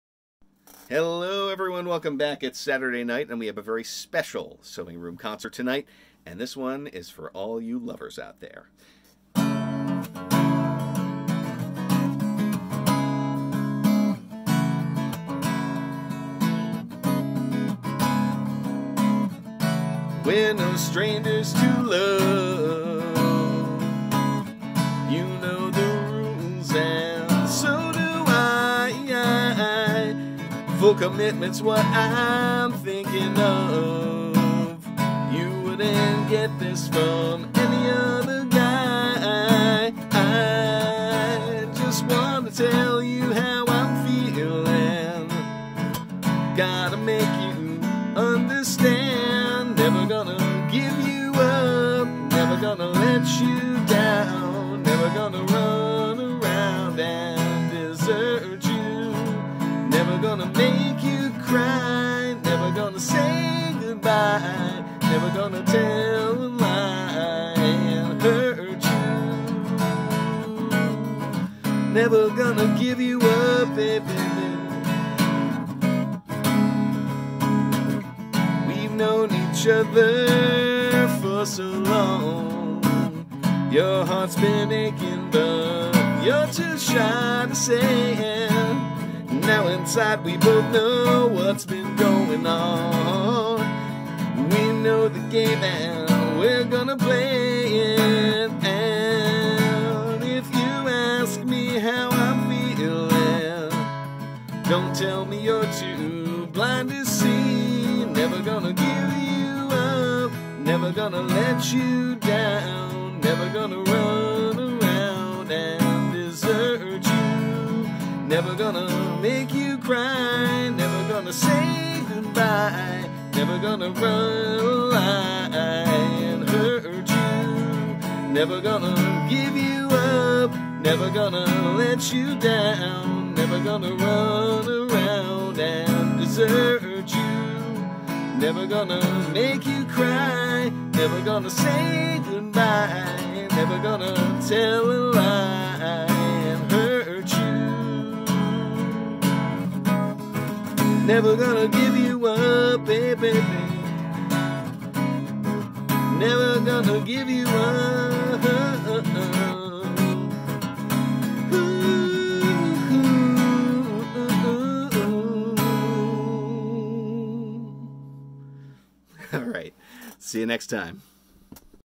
A Very Special SewingRoomConcert!
In which I sit in my sewing room and play songs on my guitar.